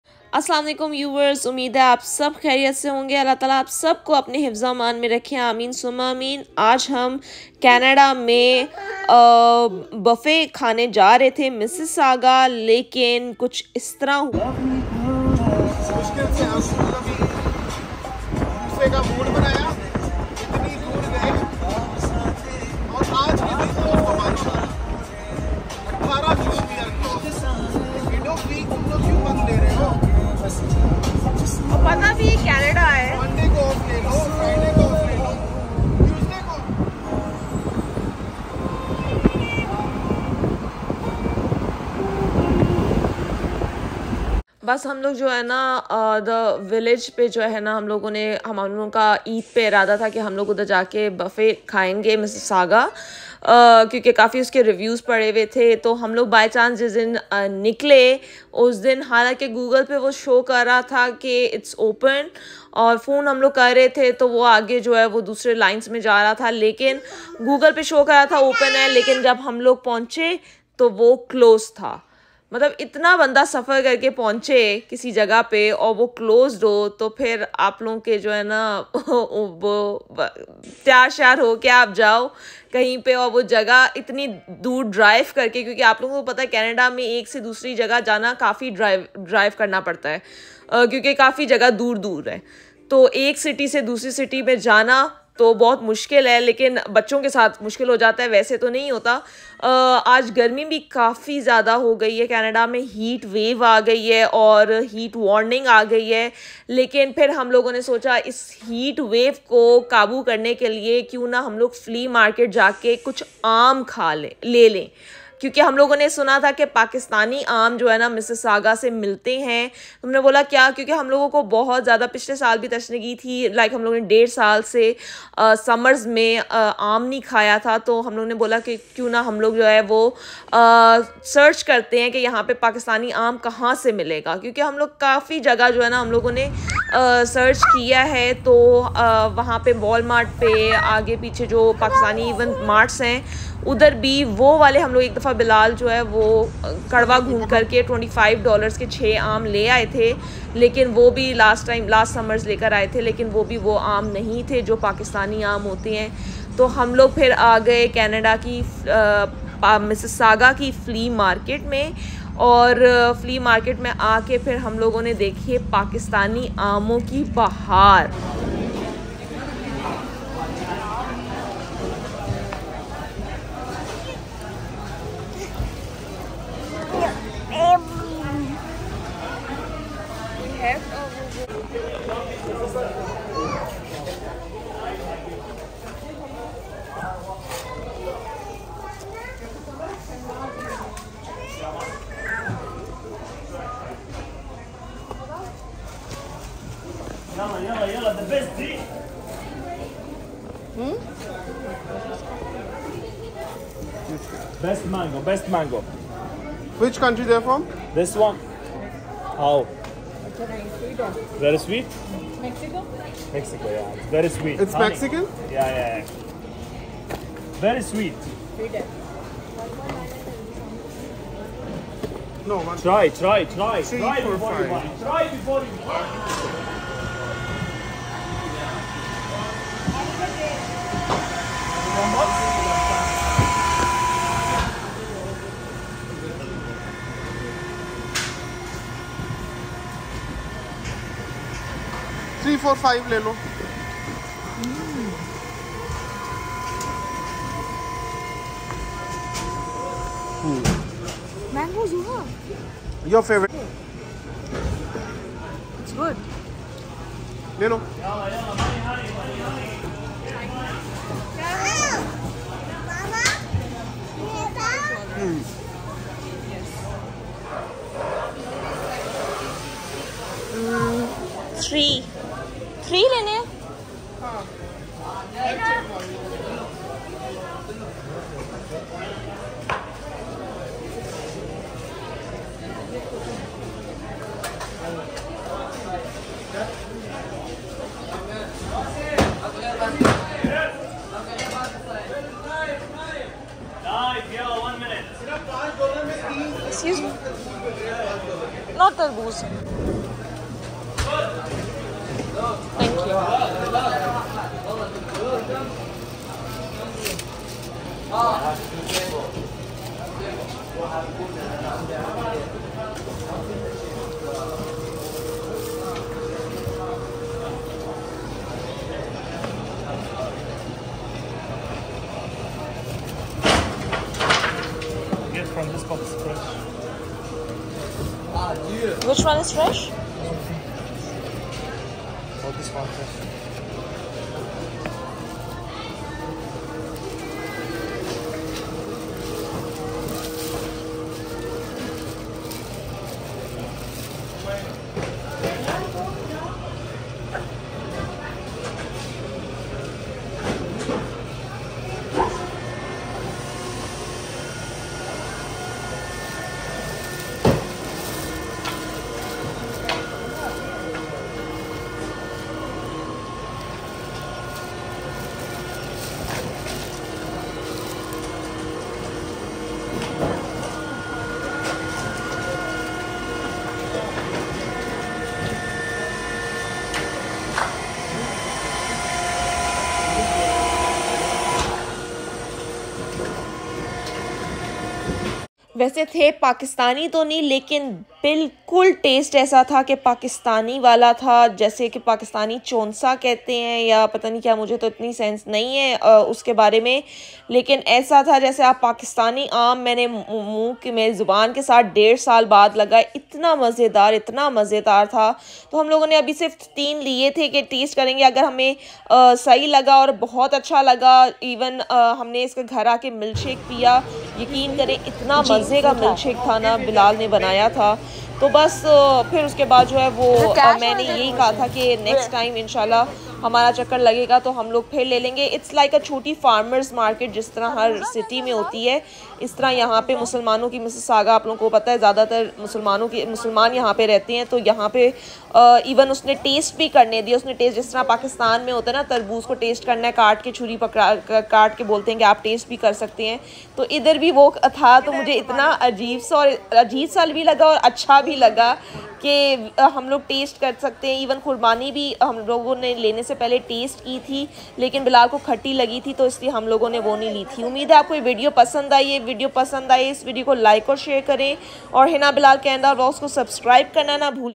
Pakistan Ki Soghat Aam Ab Canada Me 🥭🇨🇦🇵🇰| Pakistani Mangoes in Canada | Summer Special Fruit 🥭
Craving some authentic Pakistani mangoes in Canada? Look no further! Check out this video to discover where you can find this summer delicacy and satisfy your taste buds.